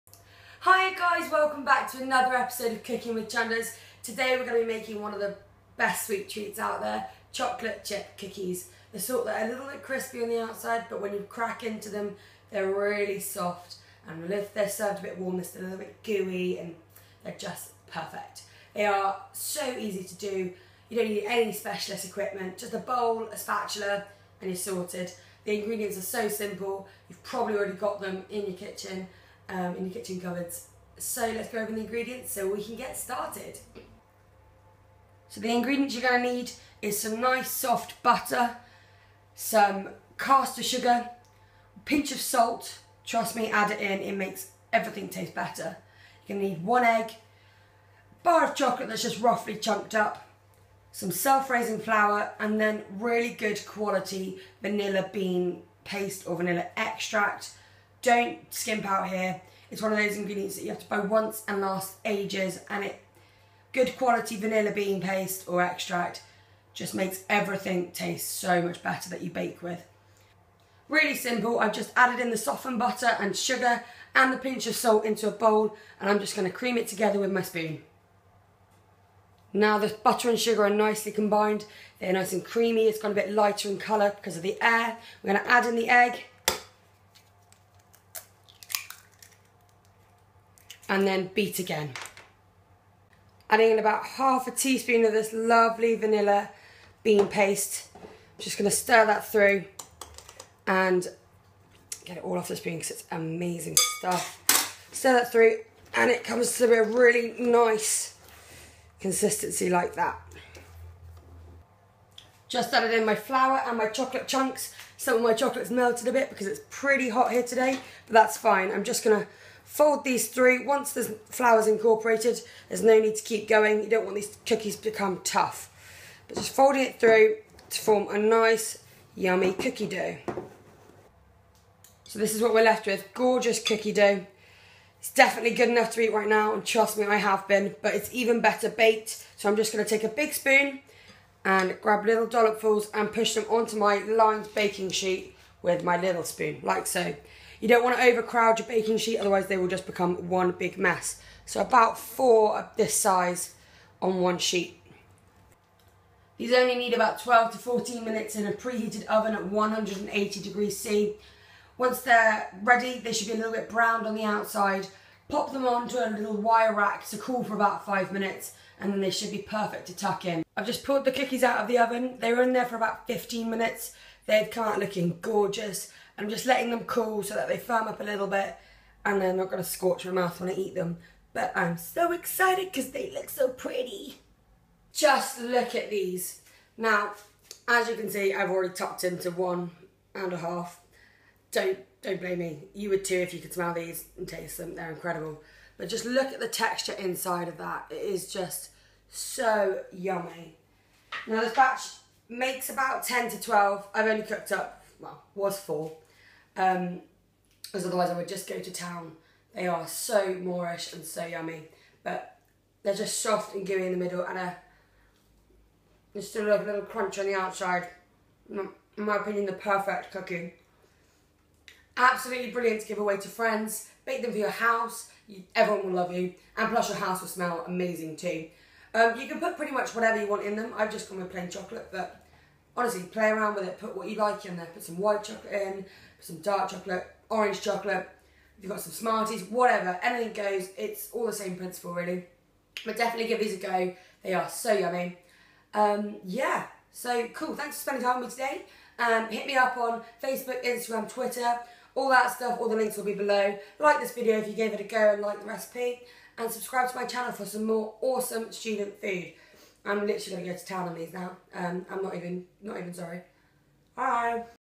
Chocolate Chip Cookie Recipe - Student Friendly Food
Hi guys, thanks for checking this video out :) everyone loves chocolate chip cookies... So here's my recipe for them. They are crunchy around the edges but soft and gooey in the middle, in other words perfect! Please give these a go, I promise you won't regret it (although your waistline might!)

Ingredients:
125g soft butter
150g caster sugar
150g self raising flour
1 egg
1/2 tsp vanilla bean paste/ extract
Pinch of salt
100g chocolate chips

Facebook.com/CookingwithChunders
Instagram.com/chunders94
Twitter.com/chunders94